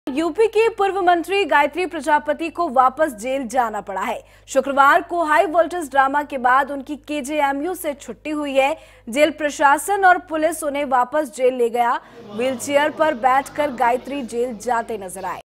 पूर्व मंत्री गायत्री प्रजापति का KGMU में हाई वोल्टेज ड्रामा, भेजे गए जेल
आखिरकार यूपी के पूर्व मंत्री गायत्री प्रजापति को वापस जेल जाना पड़ा. शुक्रवार को हाईवोल्टेज ड्रामा के बाद उनकी KGMU से छुट्टी हुई...जेल प्रशासन और पुलिस उन्हें वापस जेल ले गया...व्हील चेयर पर बैठ कर गायत्री जेल जाते देख गए..दरसल वो कई महीनों से बीमारी का बहाना बनाकर अस्पताल में इलाज करा रहे थे.